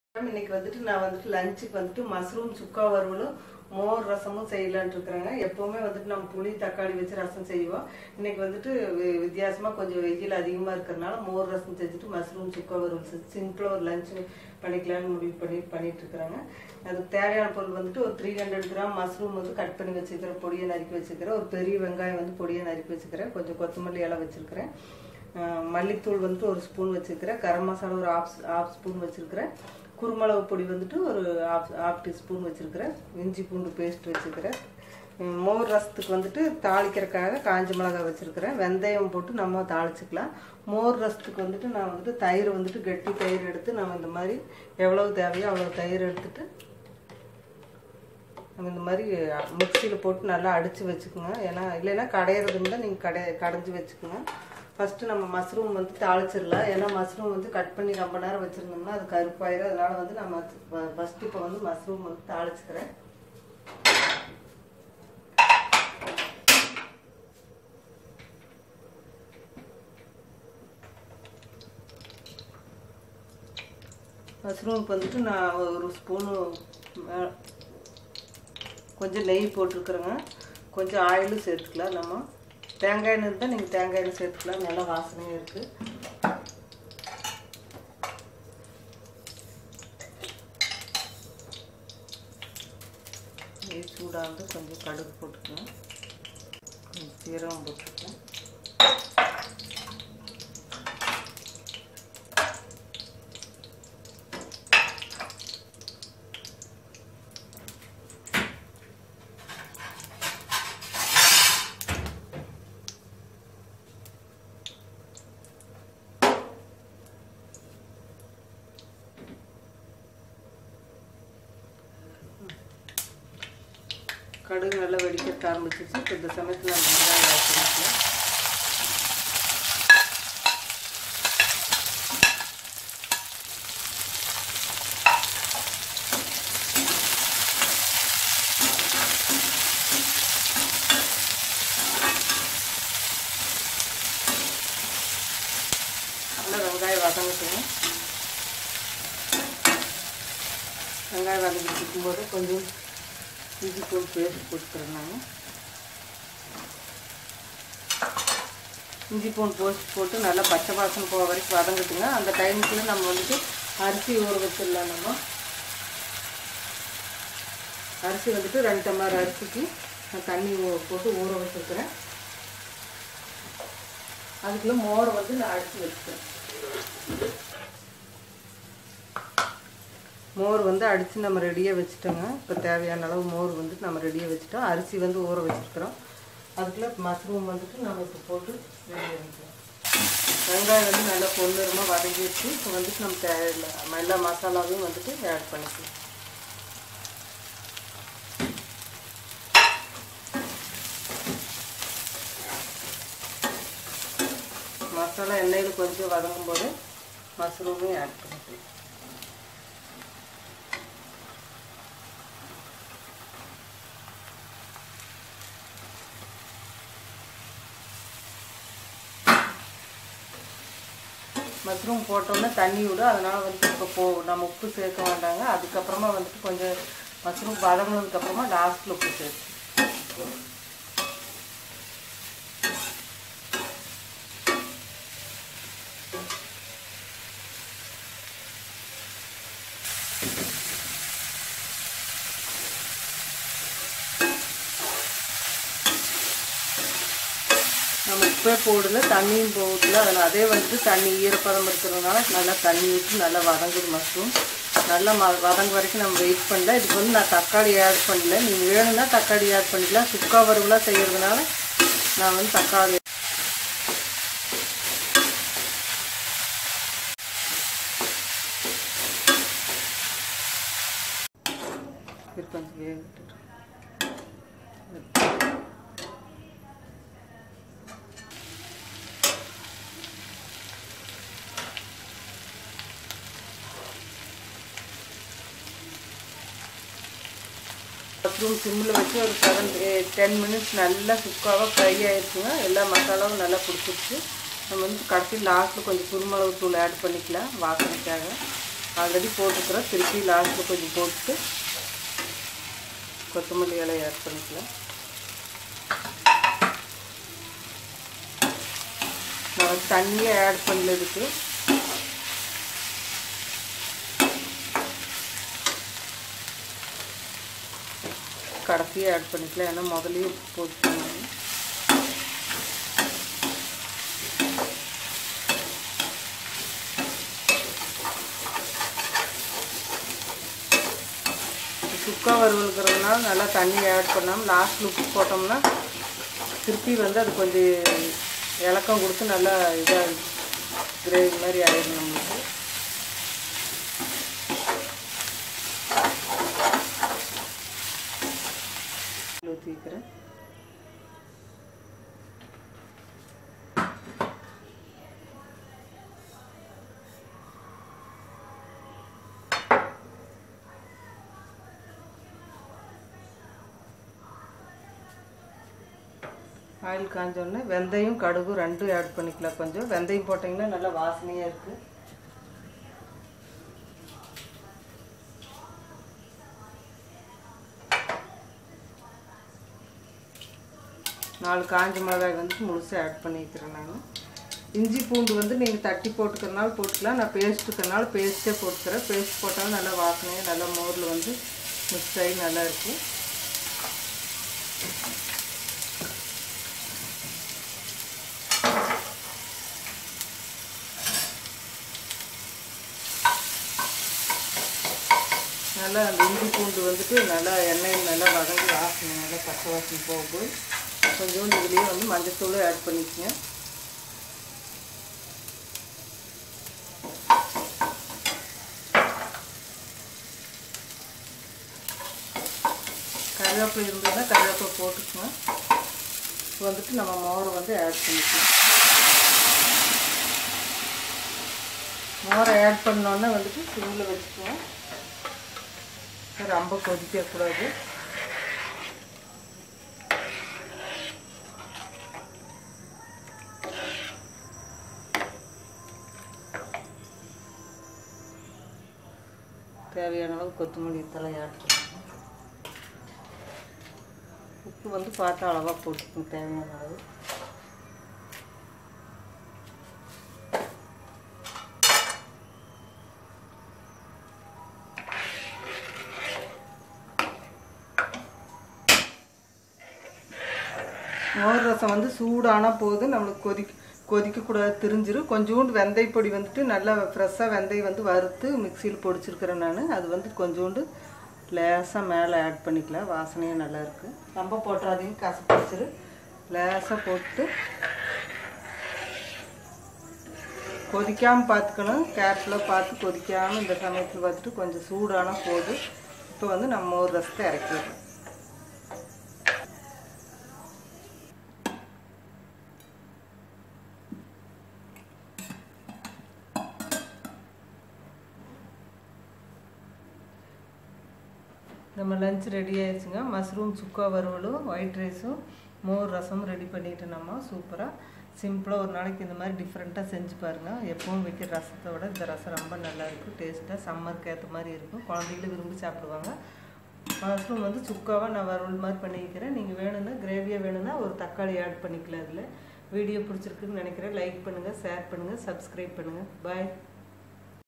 Spicy, tasty Mushroom Chukka Varuval / Video - 18
A spicy flavoured mushroom varuval
A best side dish for dosa and rice with mor kulambu